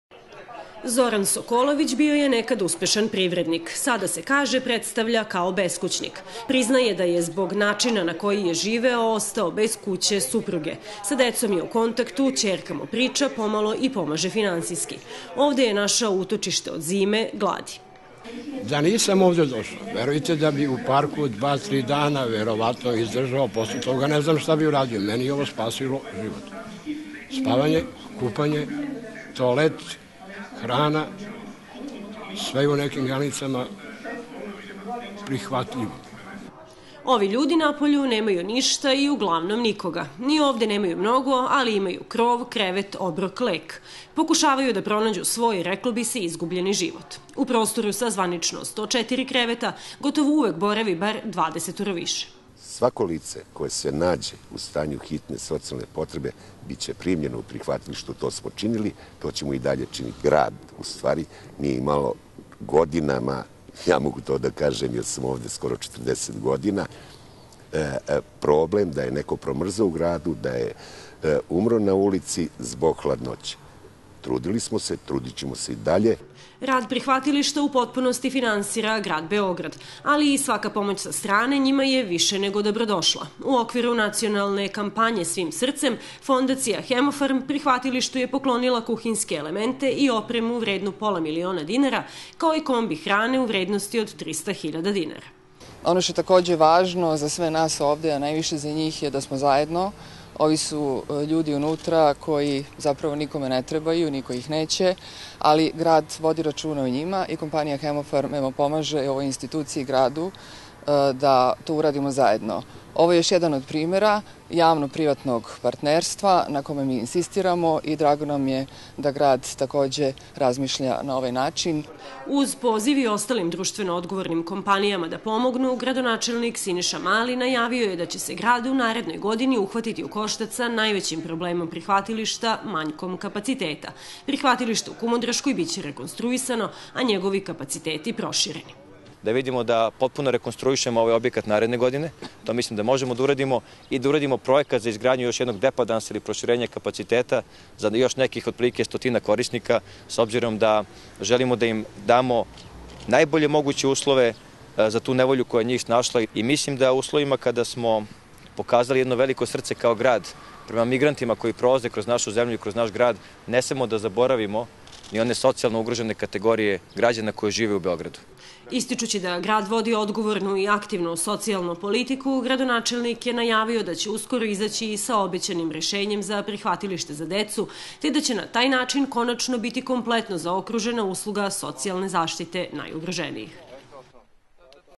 Obnova Prihvatilišta u Kumodraškoj iduće godine
Iduće godine počinje rekonstrukcija i proširenje kapaciteta Prihvatilišta za odrasla i stara lica u Kumodraškoj ulici, najavio je gradonačelnik Beograda Siniša Mali, tokom obilaska ove ustanove.